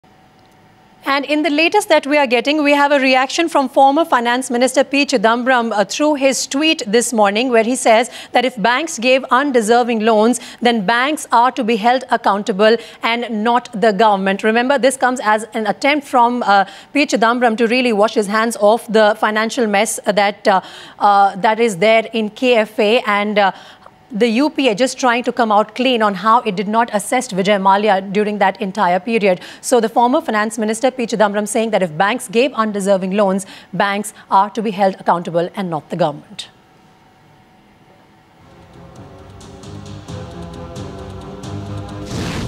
Banks Are Answerbal For Loans Says Former Finance Minister P Chidambaram
Former Finance Minister P Chidambaram on Tuesday (March 15) says that if banks gave undeserving loans, banks are to be held accountable and not the Government. Liquor baron Mallya and his grounded airline and other group companies owe around Rs 9,000 crore to 17 banks.